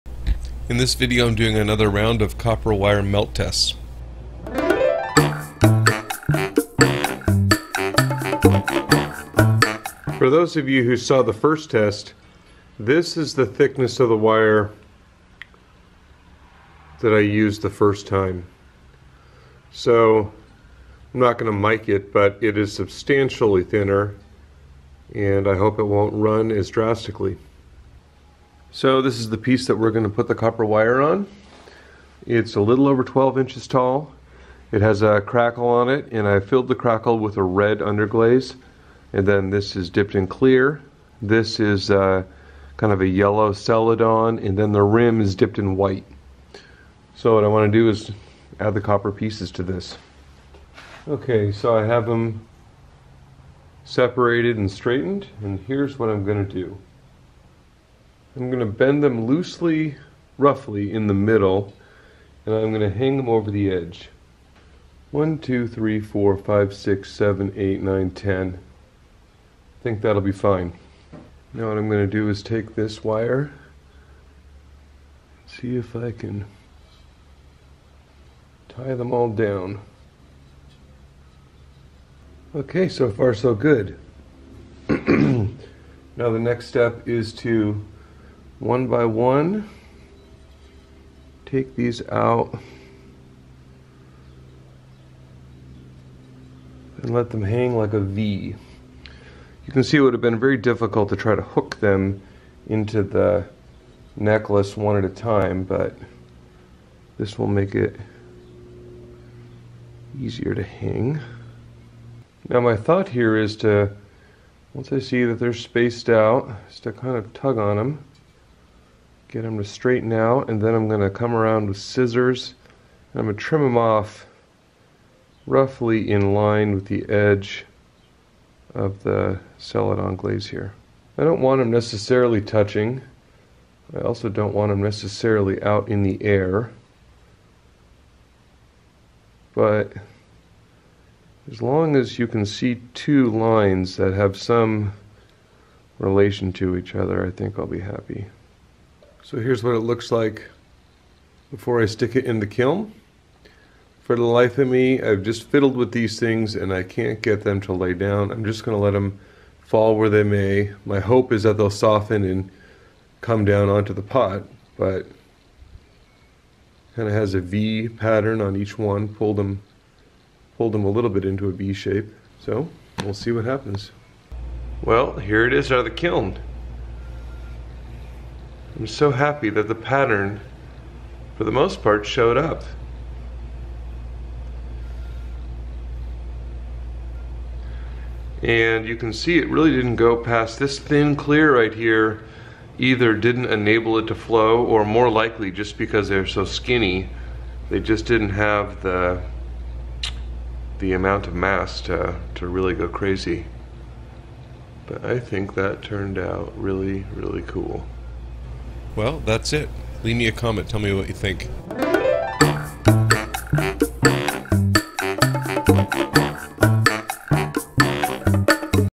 Copper Wire Glaze Test #2 - Cone 5/6 Electric Glaze Experiments
A quick video showing a glaze test using copper wire as part of the glazing process.  This is a follow-up to an earlier video using thicker wire.  This time I used thinner wire applied in a new pattern.  I am very happy with how it came out.  I hope you are inspired to do something similar.

Fired to Cone 5 in an electric kiln using B-Mix 5 clay.